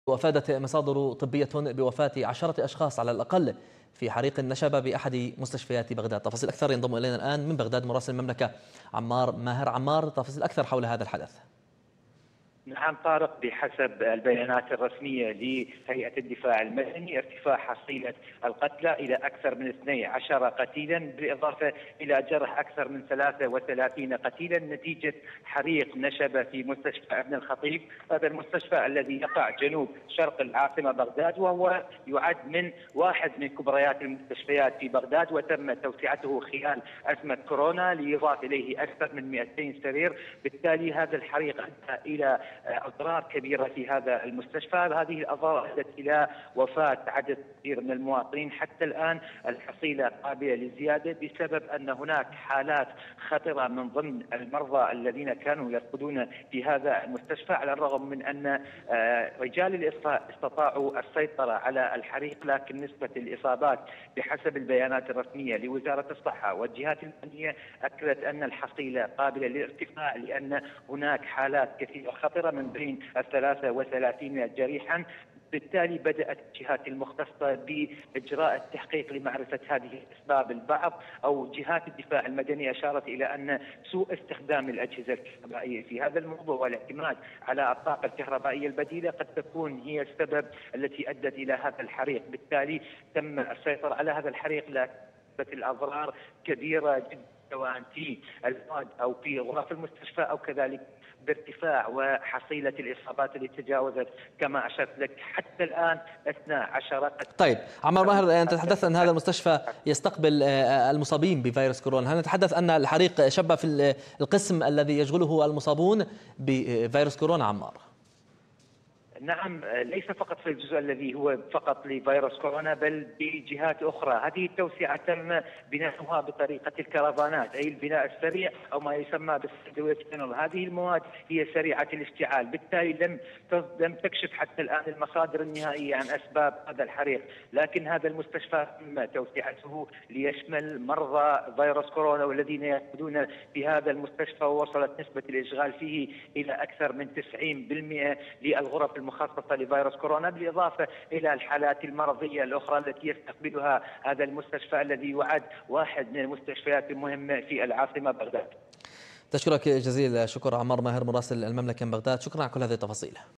مراسل المملكة: 33 جريحا في حريق المستشفى ببغداد بينها حالات حرجة جدا
مراسل المملكة عمار ماهر

هنا_المملكة